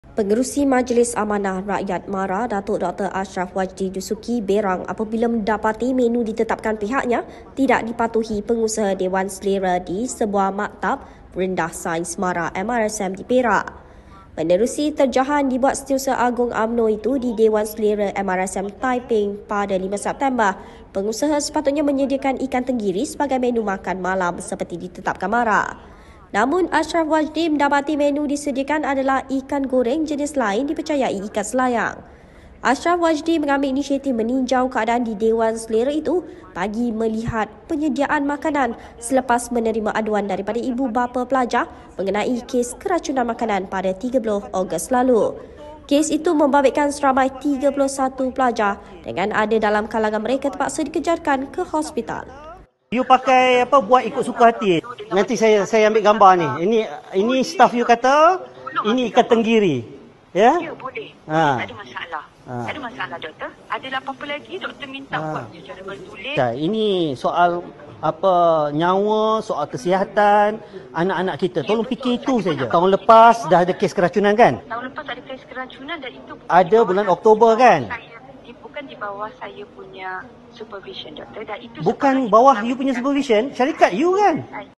Ini dikatakan ikan tenggiri?, soal Asyraf Wajdi
Pengerusi Majlis Amanah Rakyat (MARA), Datuk Dr Asyraf Wajdi Dusuki, 'berang' apabila mendapati menu ditetapkan pihaknya tidak dipatuhi pengusaha dewan selera di sebuah Maktab Rendah Sains Mara (MRSM) di Perak.